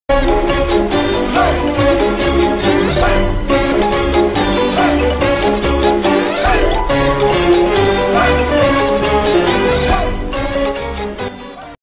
Ma nouvelle INTRO ;; pr les video gameplay / ViGa 8
salut a tous , je vous propose cette nouvelle INTRO , réalisé par moi , avec after effects , camtasia studio ;; bref ;; ENJOY